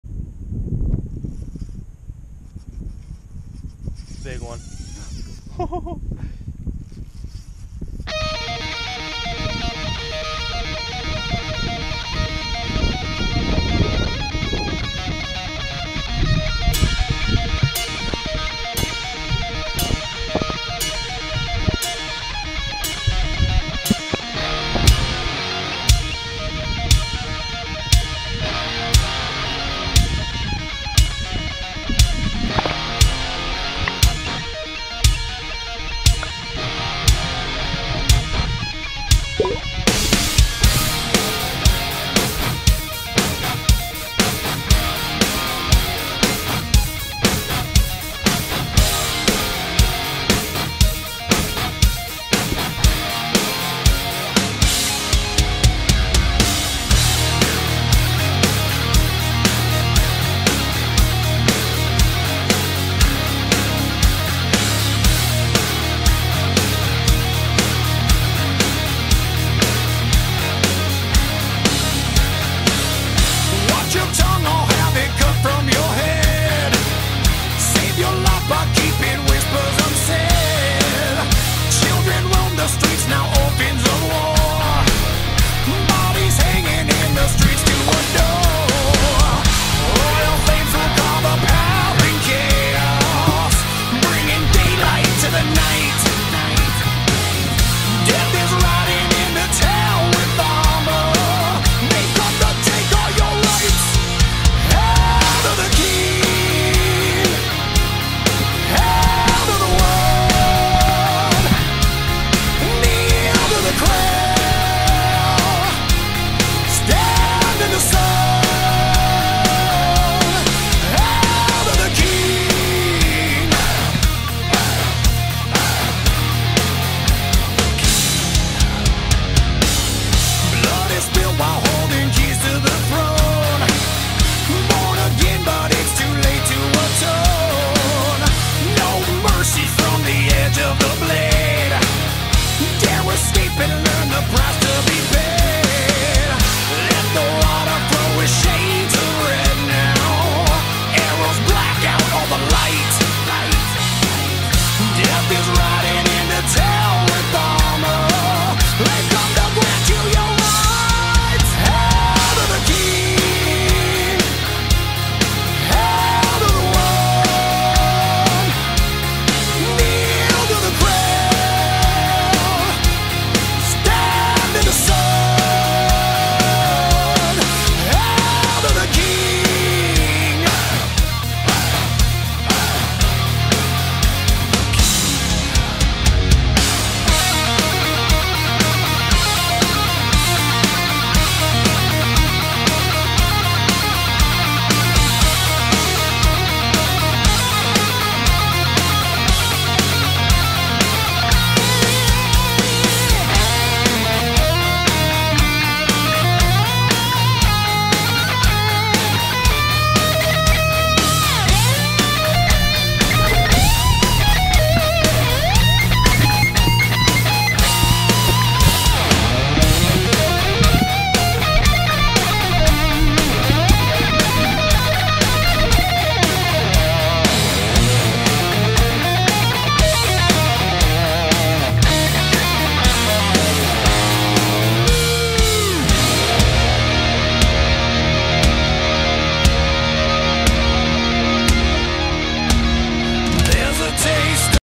Giant Rainbow Trout
Lake Aberdeen gets some giant Rainbow trout. I was lucky enough to hook into a couple.

OLYMPIC PENINSULA CHROMES